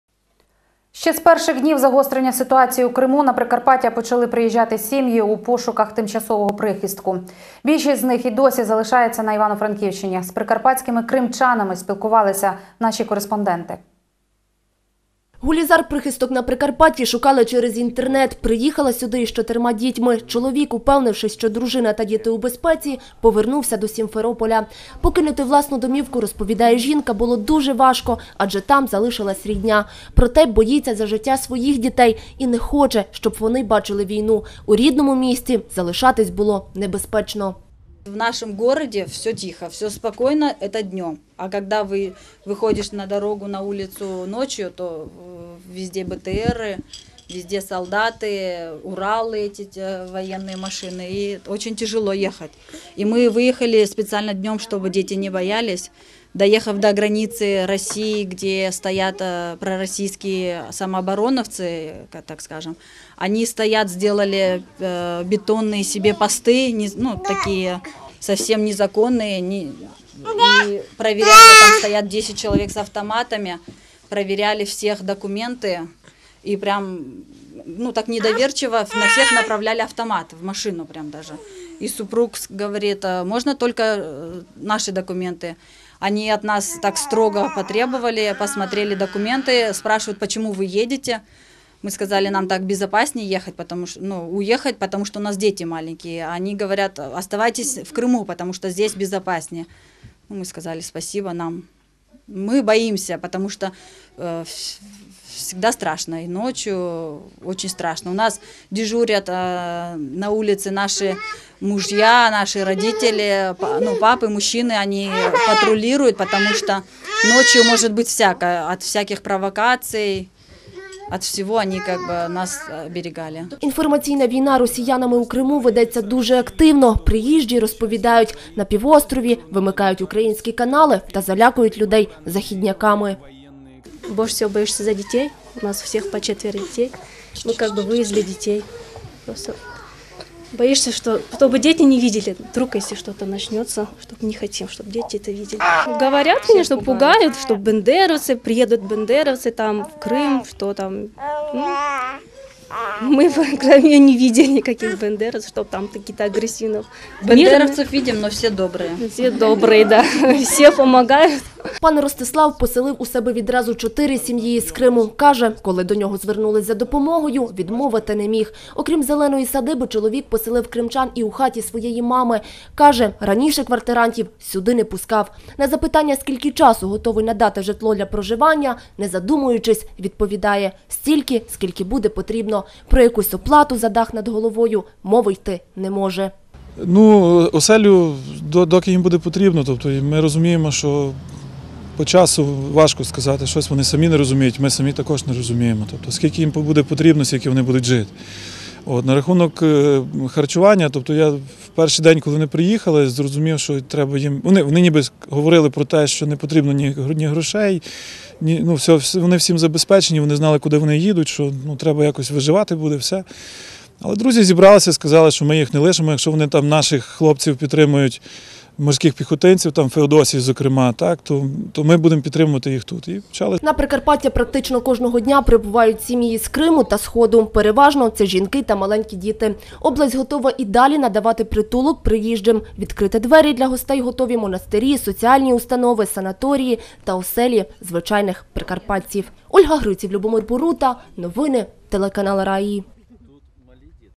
Кримські татари
Ще з перших днів загострення ситуації у Криму на Прикарпаття почали приїжджати сім`ї у пошуках тимчасового   прихістку. Більшість з них і досі залишається на Івано-Франківщині. З прикарпатськими кримчанами спілкувалися наші журналісти.